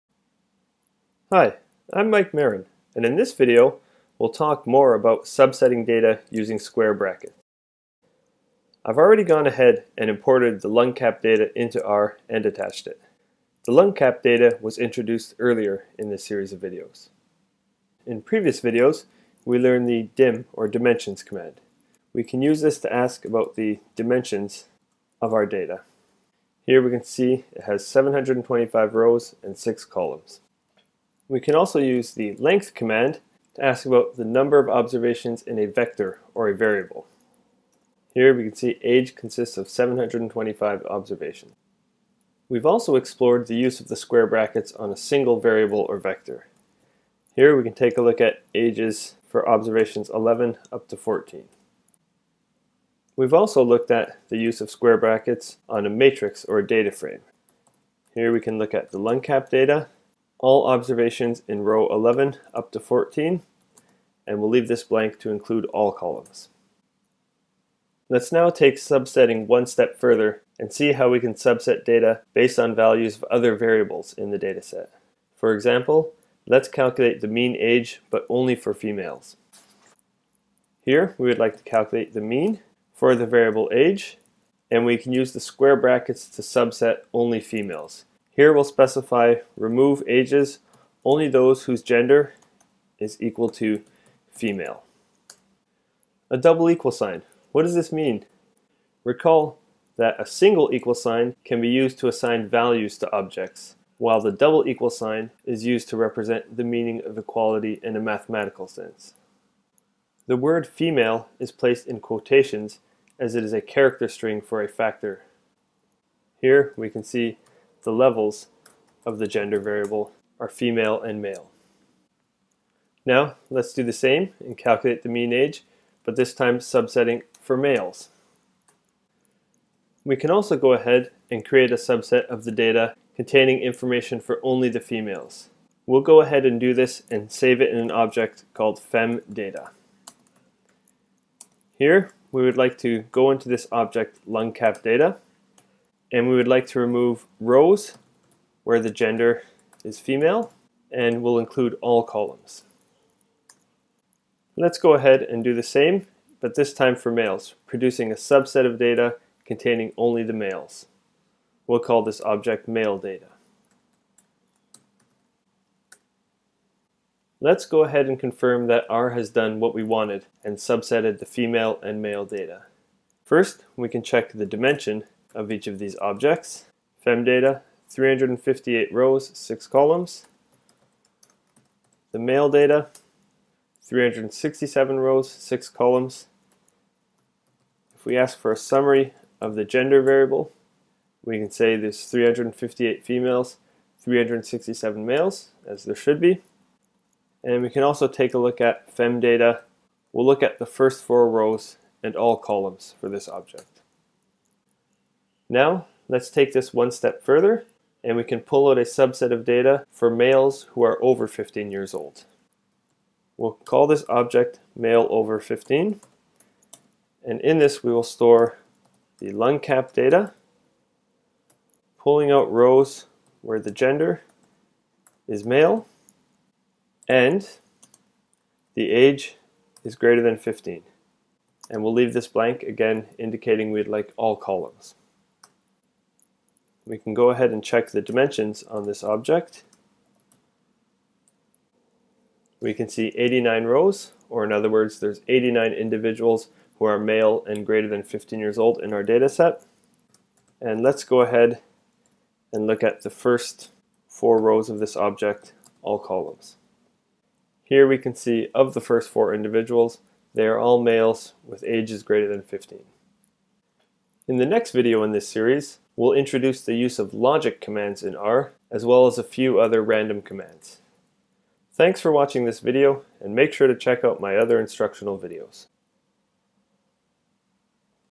Subsetting (Sort/Select) Data in R with Square Brackets | R Tutorial 1.9| MarinStatsLectures
Subsetting (Sort/Select) Data with Square Brackets and Logic Statements in R; Learn how to sort through the variables and observations and extract only the data that you need? Practice with Free (LungCap) Dataset 👍🏼Best Statistics & R Programming Language Tutorials

►► Like to support us? Either way We Thank You!

When working with a large data set, you might only be interested in a small portion of it. How do you sort through all the variables and observations and extract only the data that you need? R has several ways of sorting and selecting data in a process that is called “subsetting.” Brackets in R lets you select, or subset, data from a vector, matrix, array, list or data frame.

R’s operators can be summarized as:
• [ for subsets,
• [[ for extracting items, and
• $ for extracting by name.

In this R tutorial video, we focus on subsetting data from a vector and matrix in R.

0:00:19 How to ask R programming language the dimensions of the data? using the "dim" or dimensions function
0:00:31 How to ask R about the number of observations in a vector or variable? using the "length" function
0:00:41 How to use square brackets to extract part of the data from a vector in R
0:01:10 How to extract (subset) data from a matrix or data frame in R (step by step examples)
0:01:36 The difference between "equal sign" (=) and "double equal sign" (==) in R Software
0:03:25 How to pull out a subset of data from a dataset in R: step by step example

This video is a tutorial for programming in R Statistical Software for beginners, using RStudio.

Follow MarinStatsLectures